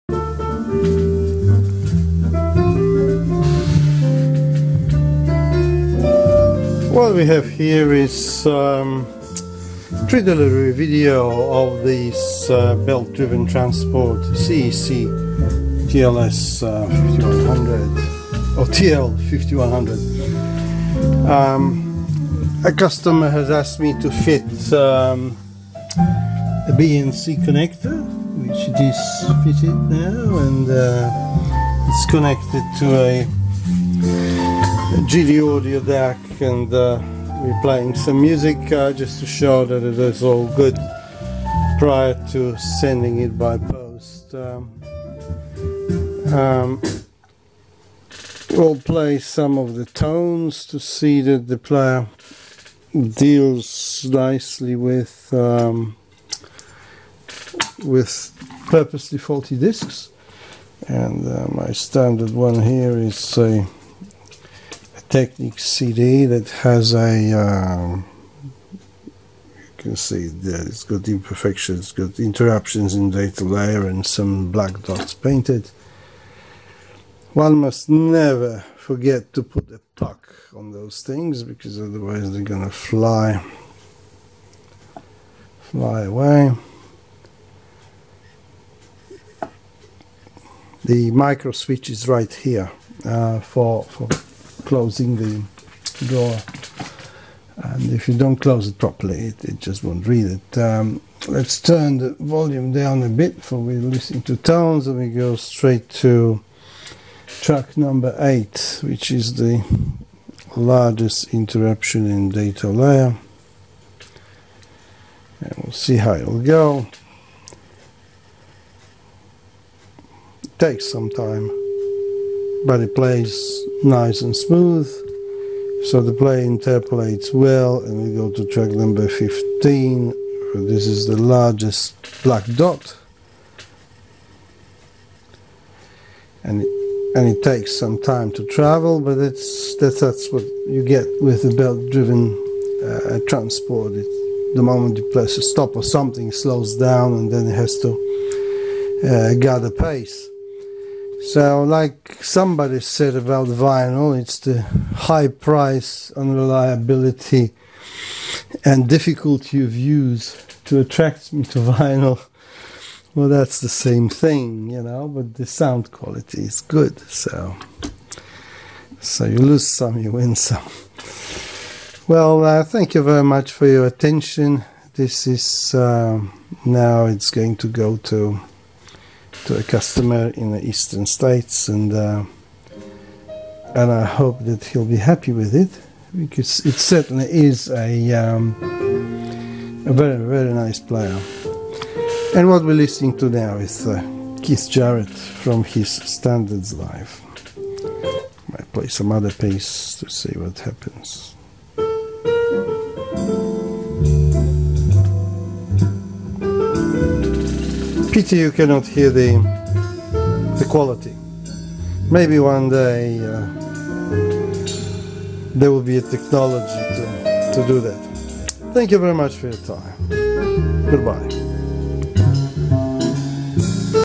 C.E.C. TL-5100 transport pre delivery video.
This is a video showing player in full working order prior to being dispatched to a customer across the country in Queensland.
BNC output connector has been fitted as requested.